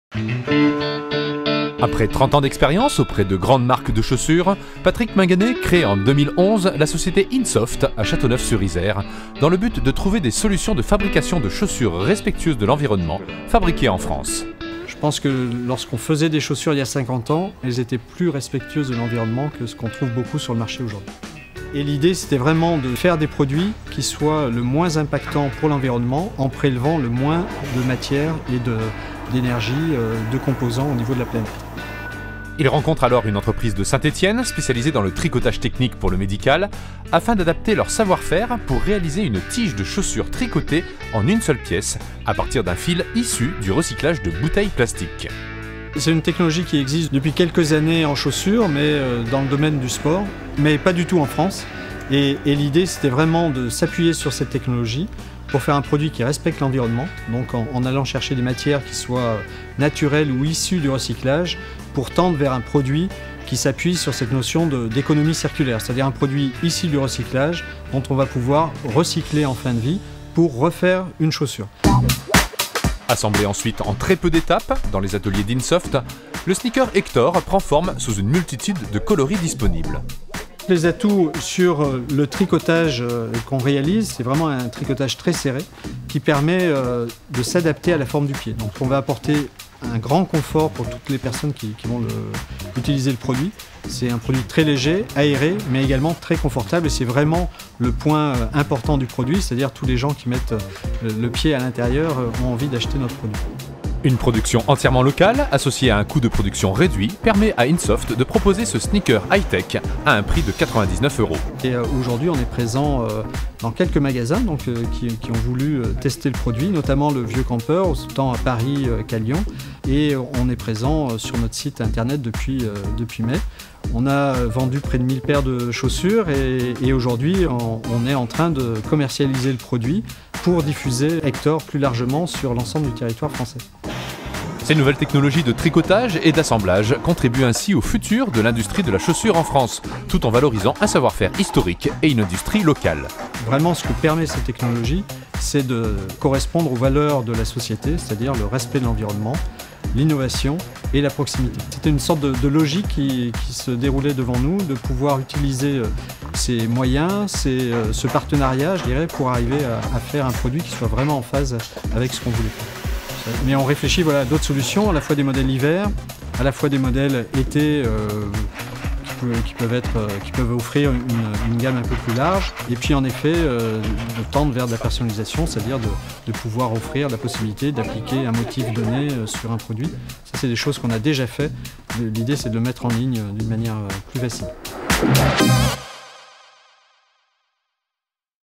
Insoft, la chaussure bio 100% française
Reportage sur la société Insoft à Valence lauréat du concours Artinov 2017.

Production : Chambre de Métiers de la Drôme
Réalisation : Manhattan Studio Productions